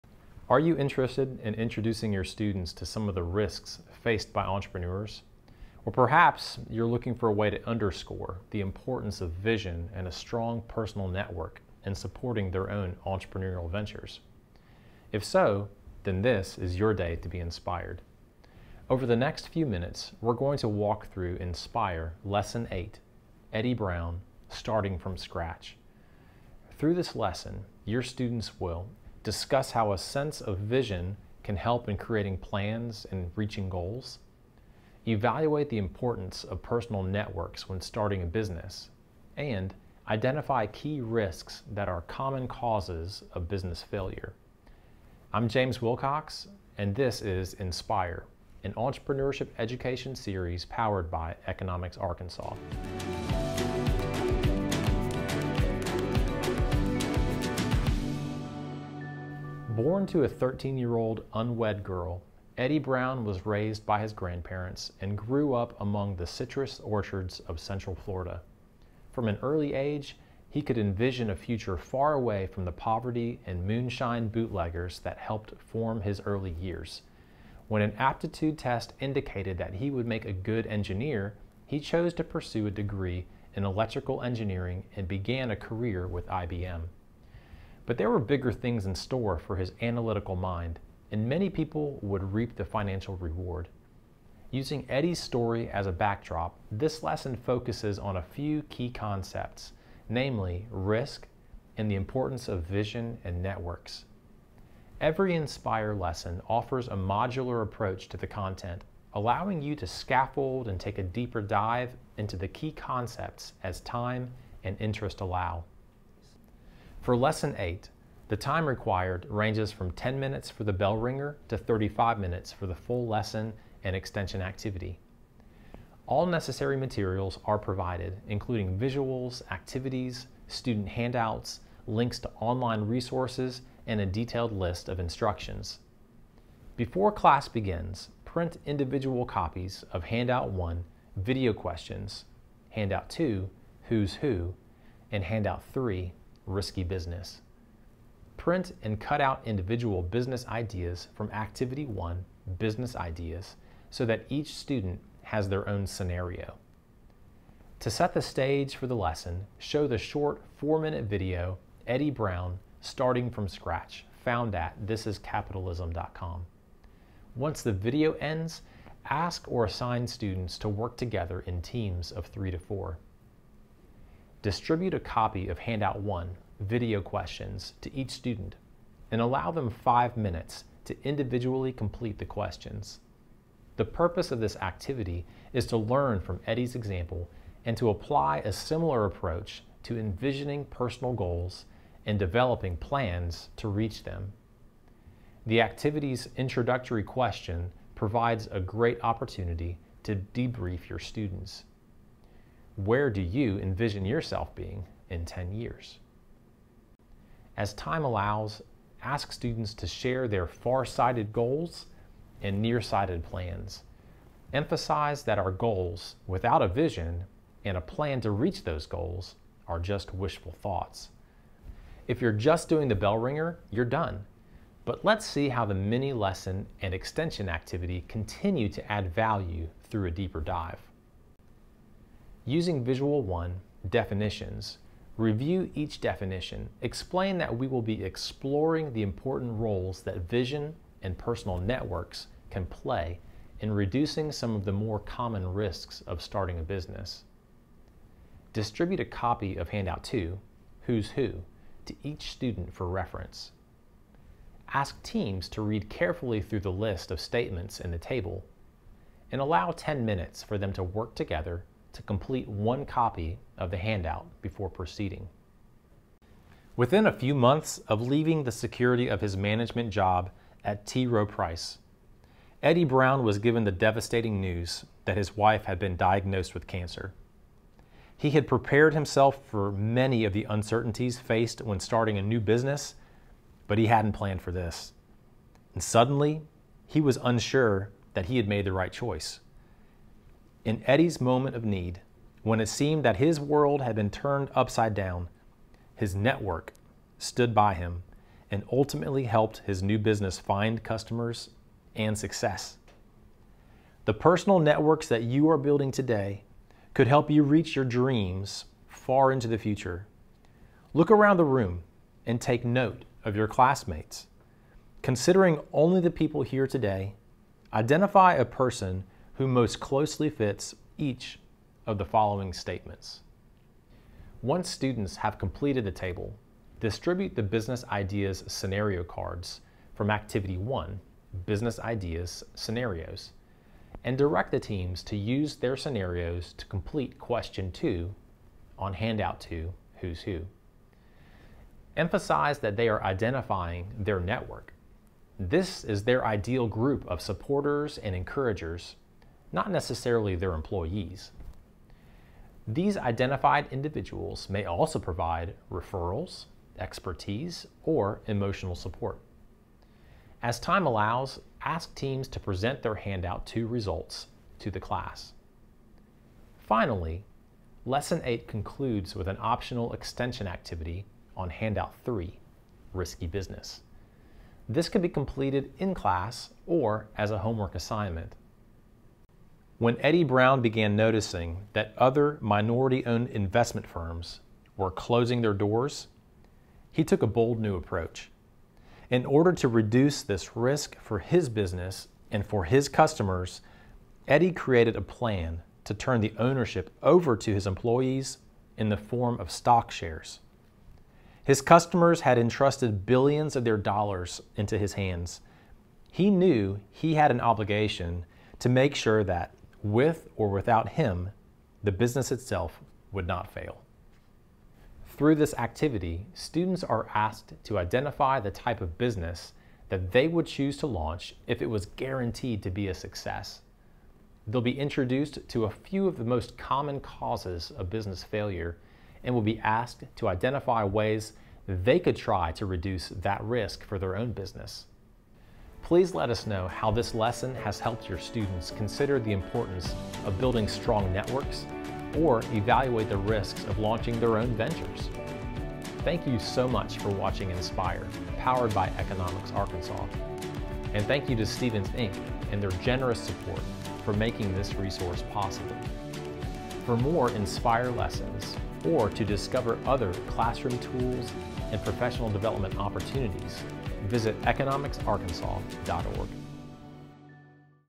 iNSPIRE This is Capitalism Demo Video 8 Eddie Brown
When an aptitude test indicated that he would make a good engineer, Eddie Brown chose to pursue a degree in electrical engineering and began a career with IBM. There were bigger things in store for his analytical mind, however, and many people would reap the financial reward.

This mini-lesson is part of the Economics Arkansas iNSPIRE! series and can be used to start a class on a number of related topics including networks, risks, and vision.